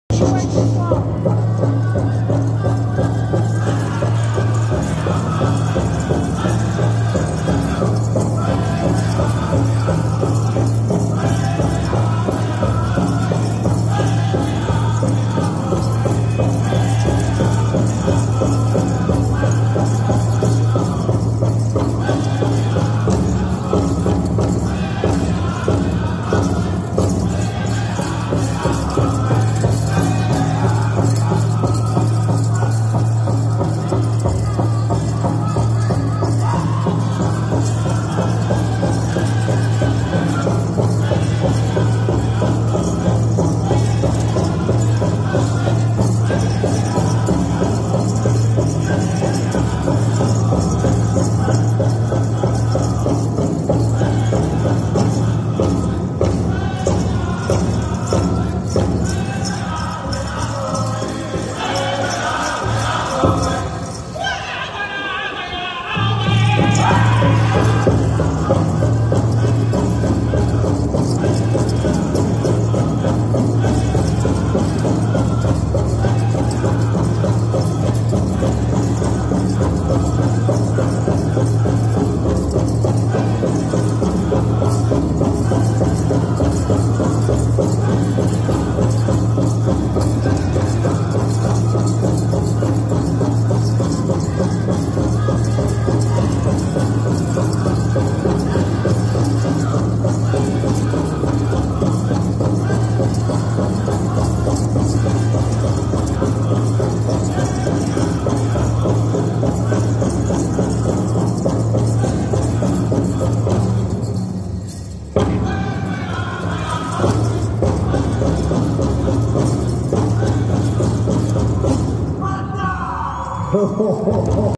Mens Fancy Dance @Washington University Pow-wow 2024
Mens Fancy in St Louis Missouri at Washington University Pow-wow 4/6/24